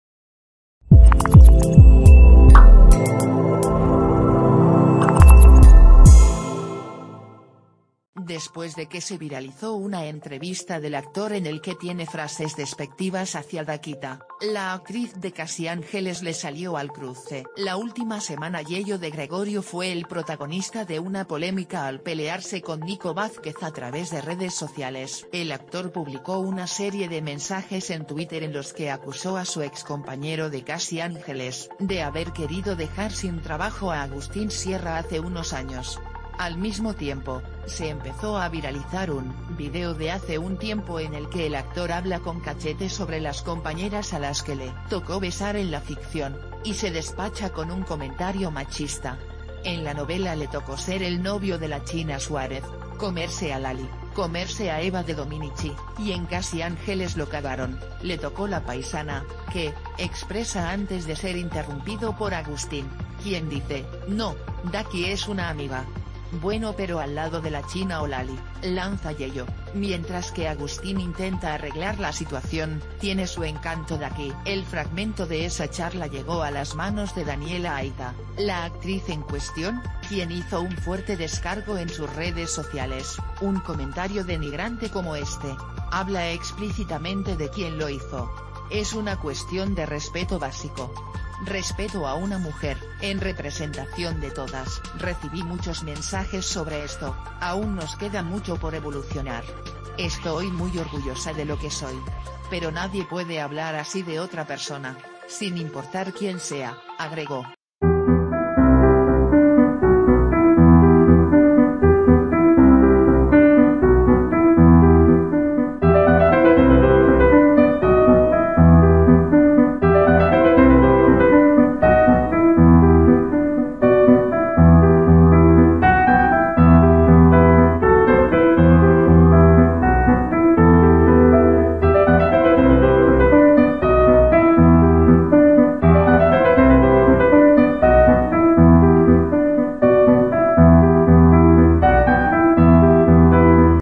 Daniela Aita después de la frase machista de Yeyo de Gregorio: "Un comentario denigrante como e...
Daniela Aita después de la frase machista de Yeyo de Gregorio: "Un comentario denigrante como este habla explícitamente de quien lo hizo" - Últimas Noticias teleshow

Después de que se viralizó una entrevista del actor en el que tiene frases despectivas hacia 'Dakyta', la actriz de Casi Ángeles le sal...